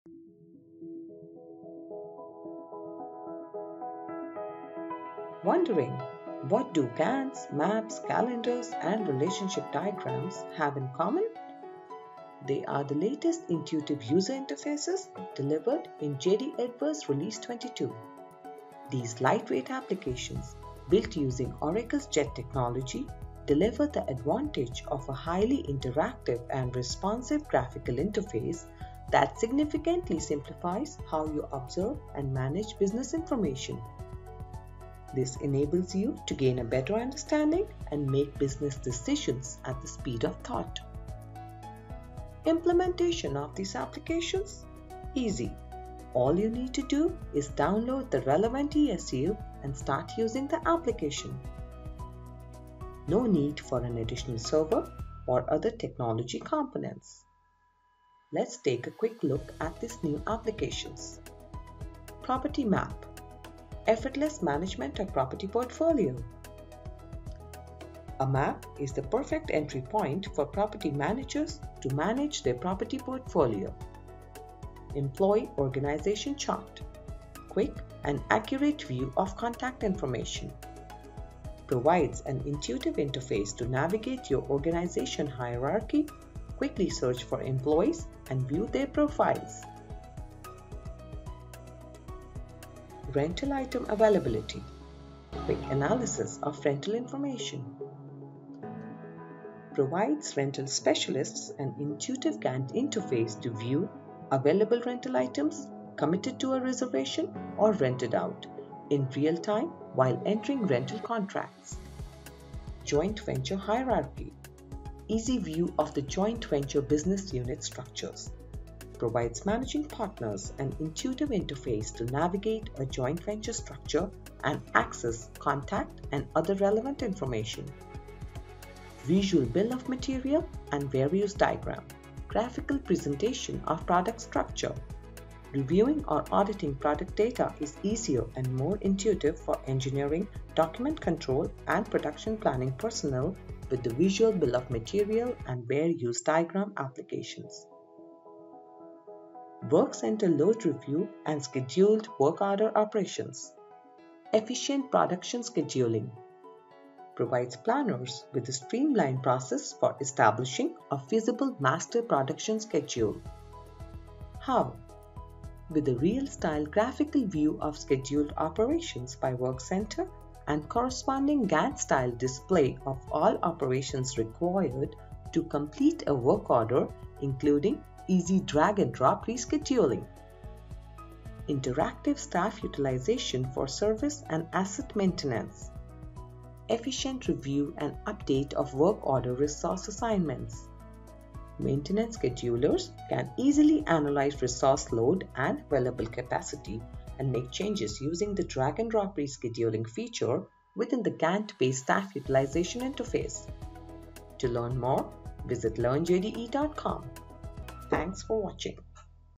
Introducing the New Intuitive and Interactive Application Interfaces
This video gives you a quick tour of the latest intuitive user interfaces delivered in JD Edwards Release 22. These intuitive and interactive graphical interfaces significantly simplify how you observe and manage business information.